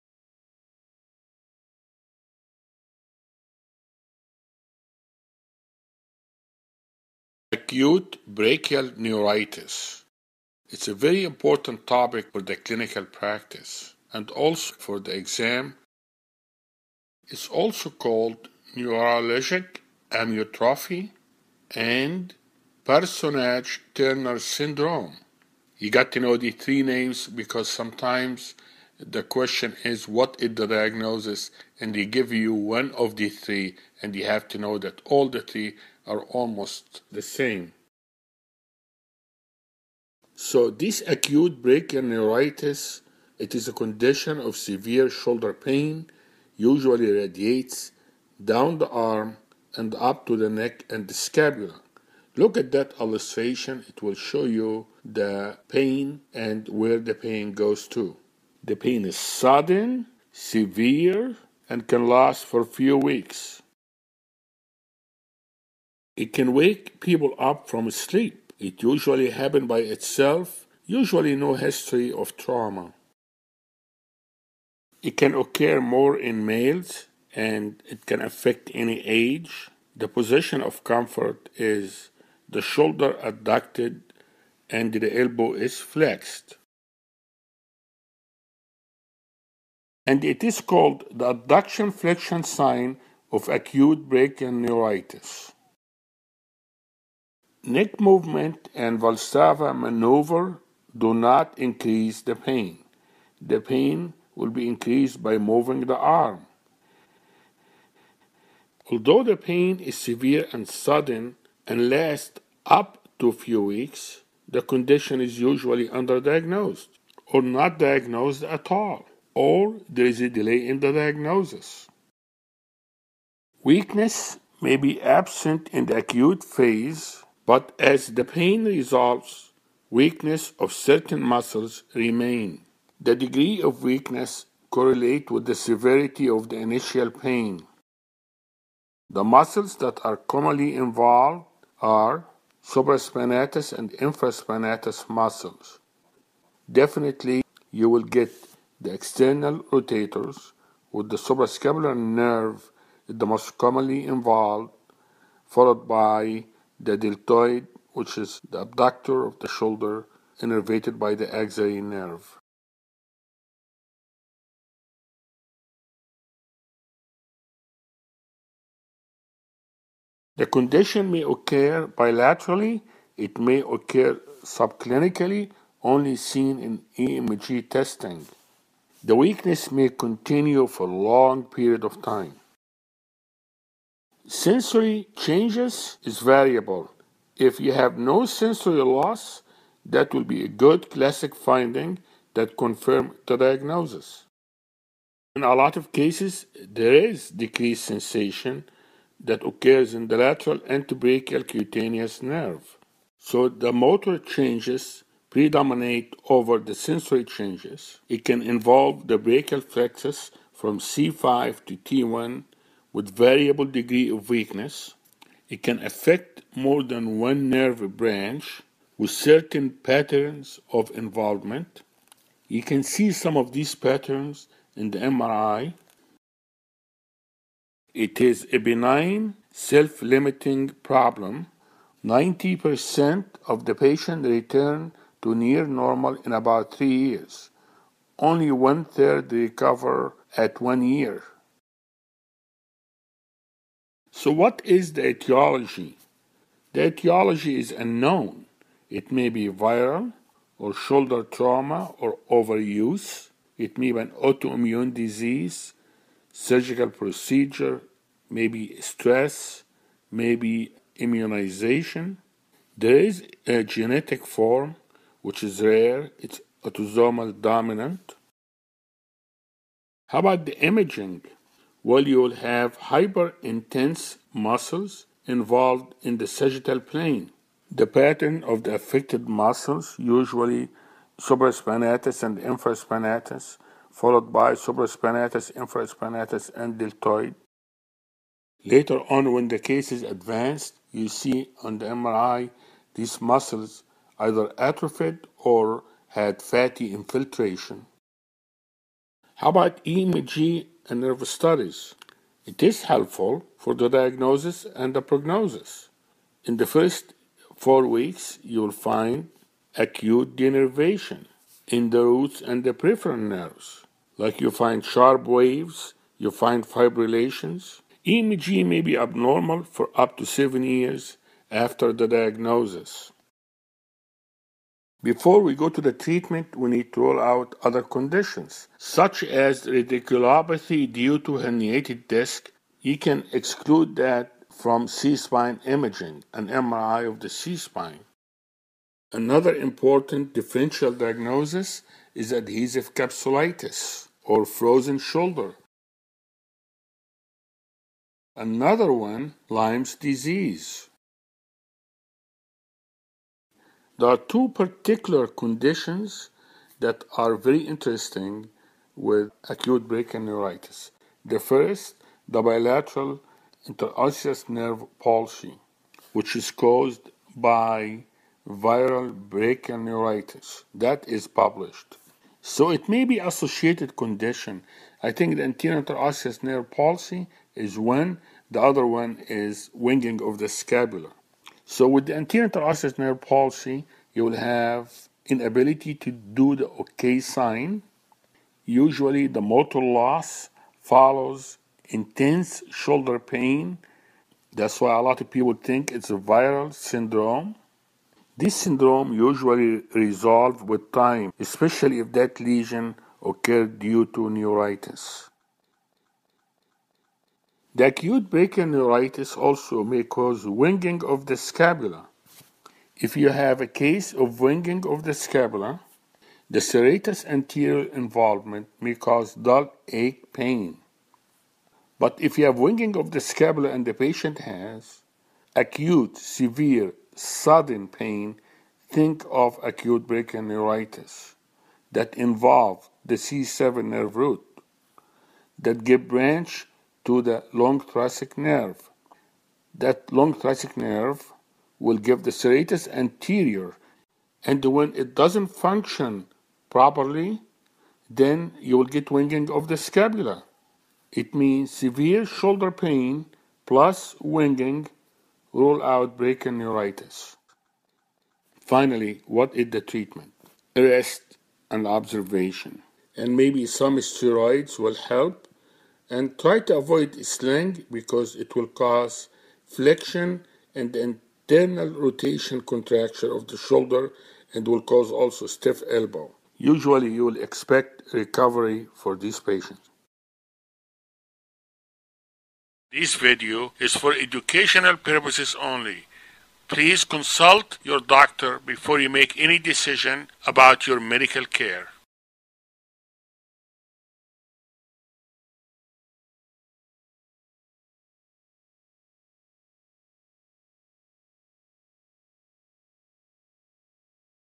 Brachial neuritis ,Parsonage turner - Everything You Need To Know - Dr. Nabil Ebraheim
Dr. Ebraheim’s educational animated video describes the Acute Brachial Neuritis.
This is a very important topic for the clinical practice and it may also appear on examinations.
Brachial neuritis is also referred to as:
• Neuralgic Amyotrophy (NA).
• Parsonage – Turner Syndrome.
It is a condition of severe shoulder pain that usually radiates down the arm and up the neck and scapula, the pain is sudden, severe and may last weeks.
This pain may wake people up from sleep, it usually happens by itself without a history of trauma.
It can occur more in males and can affect any age.
The position of comfort is the shoulder adducted with the elbow flexed, and it’s called the Adduction/ Flexion sign of Acute Brachial Neuritis.
Neck movement and Valsalva’s maneuver do not increase the pain; the pain will increase by moving the arm.
Although the pain is severe and sudden, lasting at least a few weeks, the condition is usually under diagnosed or not diagnosed at all, or there is a delay in the diagnosis.
Weakness may be absent in the acute phase, however as the pain resolve, weakness of certain muscles will remain.
The degree of weakness correlates with the severity of the initial pain.
The Muscles that are commonly involved are:
- Supraspinatus muscle, Infraspinatus muscle: the external rotators, with the suprascapular nerve is the most commonly involved.
- Deltoid muscle, which is the abductor of the shoulder, innervated by the axillary nerve.
The condition may occur bilaterally.
It may occur subclinically (only seen on IMG).
Muscle weakness may continue for a long period of time.
Sensory changes are variable:
- If there is no sensory loss, this is a classic finding that confirm the diagnosis.
- There is decreased sensation in a lot of cases.
- The lateral antibrachial cutaneous nerve is usually involved.
- The motor changes predominate over the sensory changes.
- It can involve the brachial plexus from C5-T1 with variable degree of weakness.
- It can affect more than one nerve branch with certain patterns of involvement can be seen on MRI.
- It is a benign, self limiting problem with 90% of patients returning to near normal condition in about 3 years.
- Only about 1/3 of patients will recover at about 1 year.
 The Etiology is unknown; it may be any of the following:
• Viral
• Shoulder trauma or overuse
• Autoimmune disease
• Stress
• Immunization
• Genetic: there is a genetic form, which is rare, its autosomal dominant.
Imaging:
- Hyperintense muscles involved in the sagittal plane (supraspinatus, infraspinatus, deltoid)
- In advanced cases, the muscles will either be atrophied or have fatty infiltration.
EMG and Nerve Studies:
- Helpful for the diagnosis and the prognosis.
- In the first 4 weeks there will be acute denervation in the roots and the peripheral nerves.
- EMG may be abnormal for up to 7 years after the diagnosis.
 Differential Diagnosis:
- Rule out other conditions, such as radiculopathy from a herniated disc.
- This can be excludes by imaging the cervical spine.
- Adhesive capsulitis or frozen shoulder.
- Lyme disease.
The 2 particular conditions that are very interesting with acute brachial neuritis (caused by the Brachial neuritis):
1- Bilateral Interosseous nerve palsy: which is caused by viral brachial neuritis (published data), patient will lose the ability to do the O.K. sign, usually the motor loss follows intense shoulder pain, usually resolves with time especially if that lesion occurred due to neuritis.
2- Winging of the Scapula:
- If you have a case of winging of the scapula: Serratus anterior muscle involvement may cause dull aches and pain.
- But if you have winging of the scapula and the patient has: Acute, sudden severe pain then think of acute brachial neuritis that involves the C7 nerve roots.
- C7 nerve root gives the long thoracic nerve which innervates the serratus anterior muscle.
- This means that if the patient has:
Severe shoulder pain + Winging of the scapula: then you have to rule out Brachial Neuritis.
Treatment:
• Rest
• Observation
• Steroid injection
• Avoid using a sling as it will cause flexion and internal rotation contracture of the shoulder, and will cause also stiff elbow.
Usually you will expect recovery for these patients.